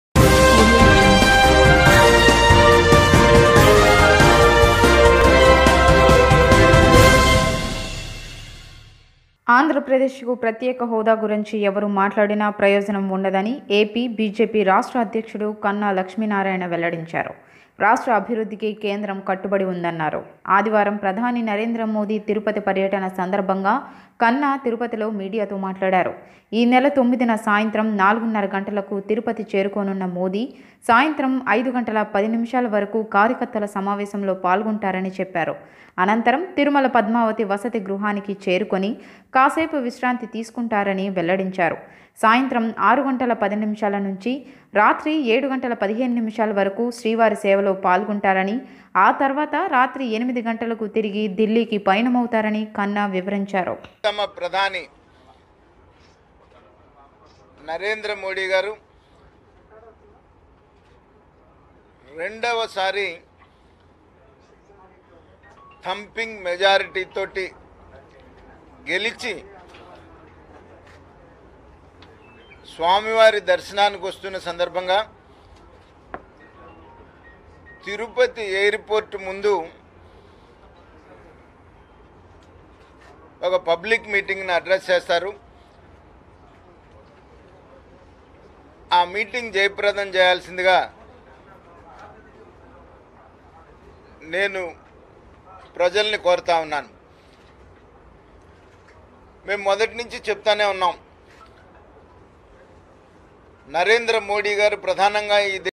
ప్ర‌ధాని మోదీ ప‌ర్య‌ట‌న‌ని వివ‌రించిన క‌న్నా
ఆంధ్రప్రదేశ్‌కు ప్రత్యేక హోదా గురించి ఎవరు మాట్లాడినా ప్రయోజనం ఉండదని AP  BJP  రాష్ట్ర అధ్యక్షుడు కన్నా లక్ష్మీనారాయణ వెల్ల‌డించారు.   రాష్ట్ర అభివృద్ధికి కేంద్రం కట్టుబడి ఉందన్నారు.  ఆదివారం ప్రధాని నరేంద్ర మోదీ తిరుపతి పర్యటన సందర్భంగా కన్నా తిరుప‌తిలో  మీడియాతో మాట్లాడారు.  ఈ నెల 9న సాయంత్రం 4.30గంటలకు తిరుపతి చేరుకోనున్న మోదీ.. సాయంత్రం 5.10 గంటల వరకు కార్యకర్తల సమావేశంలో పాల్గొంటారని చెప్పారు. అనంతరం తిరుమల పద్మావతి వసతిగృహానికి చేరుకొని కాసేపు విశ్రాంతి తీసుకుంటారని వెల్లడించారు.  సాయంత్రం 6.10 గంటల నుంచి రాత్రి 7.15 గంటల వరకు శ్రీవారి సేవలో పాల్గొంటారని,  ఆ తర్వాత రాత్రి 8 గంటలకు తిరిగి దిల్లీకి పయనమవుతారని కన్నా వివరించారు.